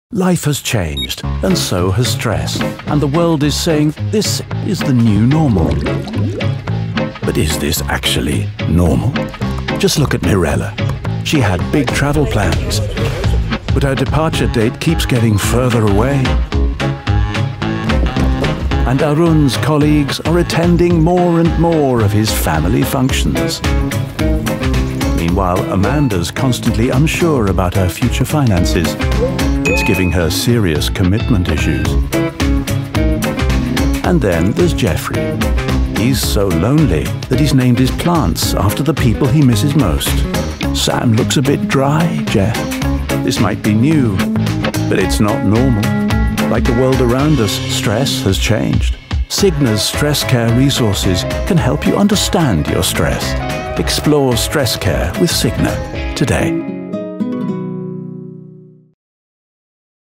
New, but not 'normal' - manage your stress with Cigna Stress Care
Life has changed since the pandemic, and show has stress. Cigna's stress care resources can help you take control.

Whether you’re struggling to separate work and home life, feeling cut off from loved ones, worried about your future finances, or unable to get where you need to be – we’re all dealing with new stress. And though this may be new, it’s not normal.
Cigna has a curated range of Stress Care resources that can help you check in on your stress so you don’t have to face it alone.  Explore Stress Care today with Cigna